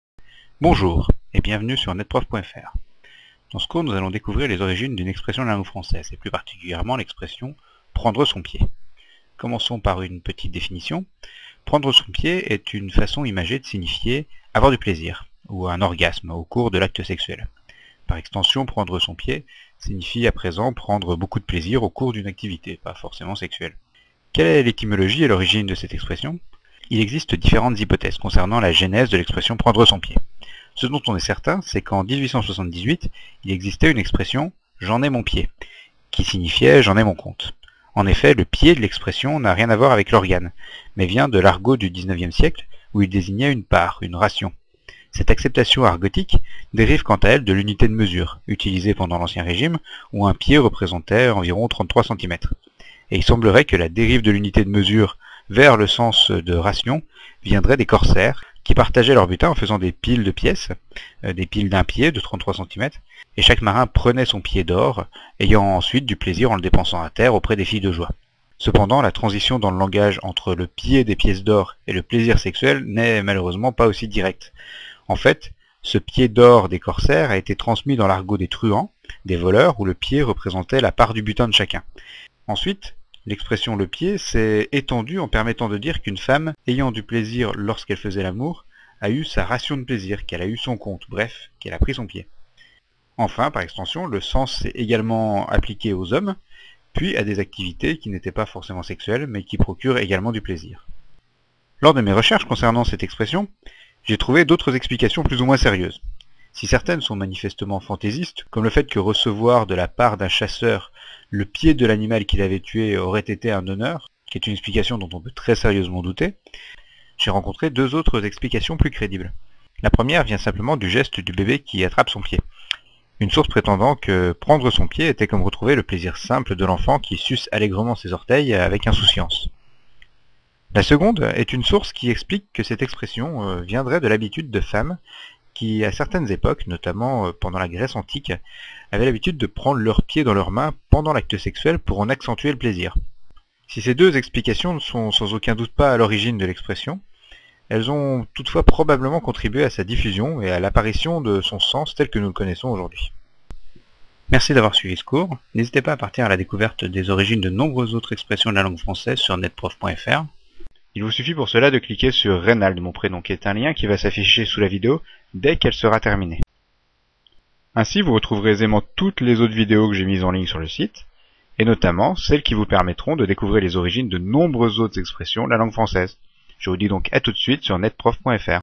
Aux origines d'une expression - 016. Prendre son pied
Cours netprof.fr de Français / Expressions
Prof : Reynald